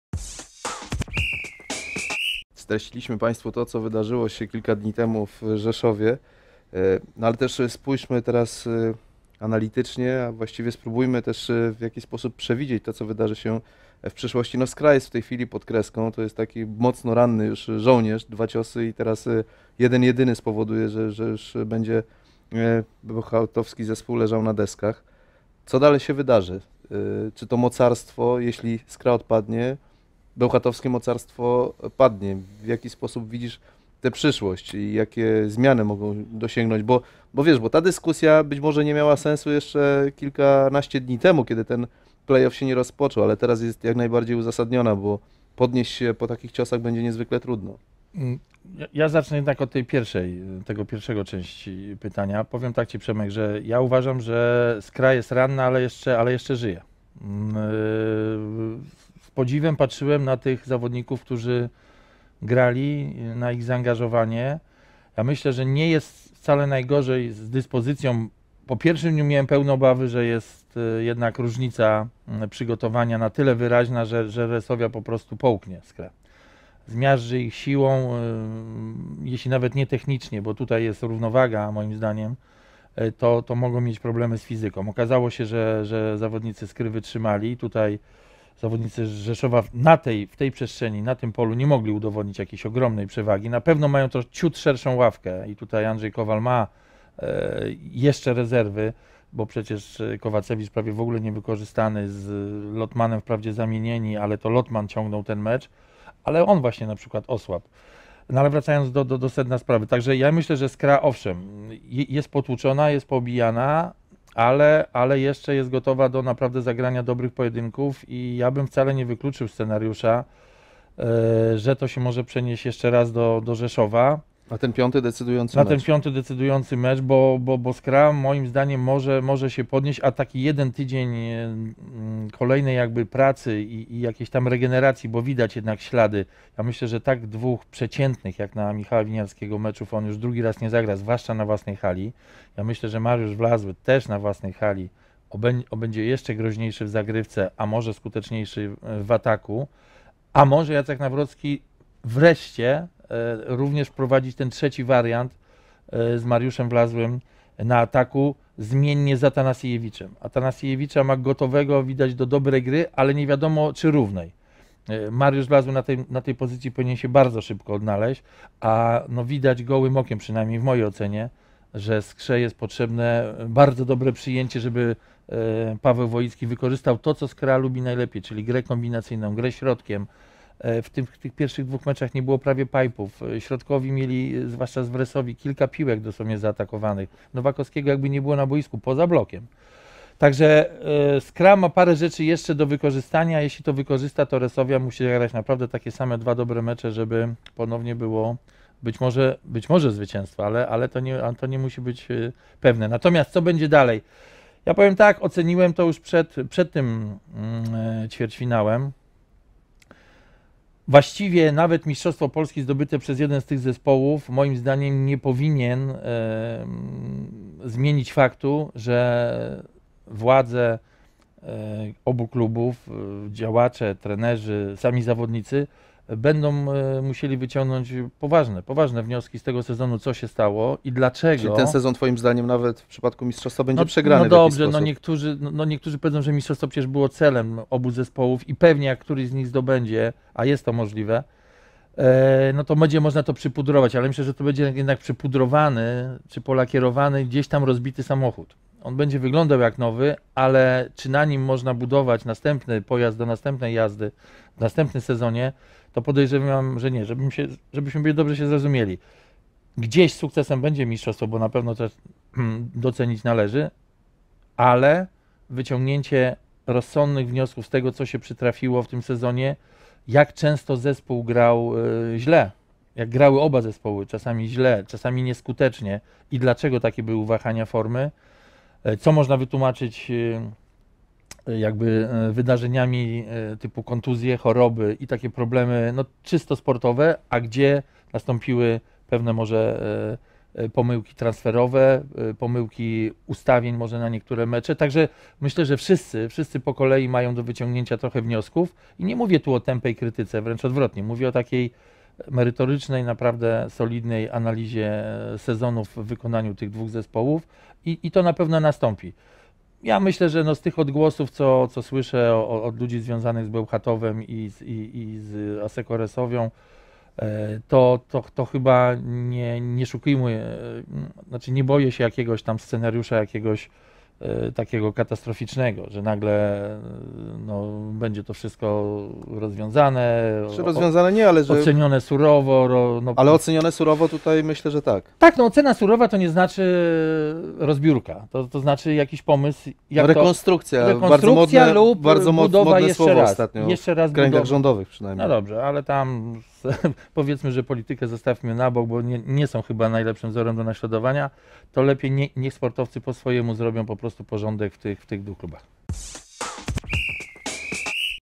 Drzyzga: Skra jest ranna, ale jeszcze żyje
Sport.pl prezentuje: Mistrzostwo Polski zdobyte przez którykolwiek z tych zespołów, nie zwolni działaczy, trenerów i zawodników od oceny tego, co się wydarzyło. To i tak będzie polakierowany, rozbity samochód. Przypomnijmy sobie, jak źle grały te drużyny w trakcie sezonu na kilku frontach, ile było pomyłek transferowych, złego ustawienia zespołu, itd. Więcej wideo na